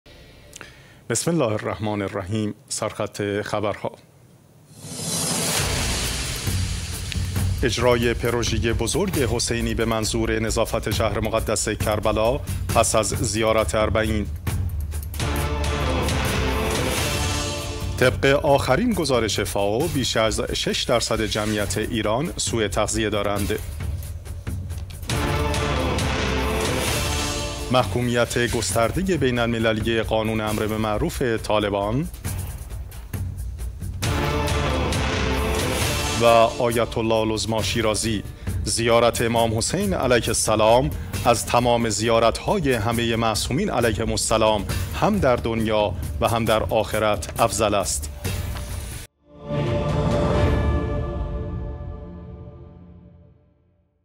سر خط خبر ها
-  اجرای پروژه بزرگ حسینی به منظور نظافت شهر مقدس کربلا پس از زیارت اربعین

-  طبق آخرین گزارش فائو، بیش از شش درصد جمعیت ایران سوءتغذیه دارند

- محکومیت گسترده بین‌المللی قانون امر به معروف طالبان

 - آیت الله العظمی شیرازی: زیارت امام حسین علیه السلام از تمام زیارت های همه معصومین علیهم السلام هم در دنیا و هم در آخرت افضل است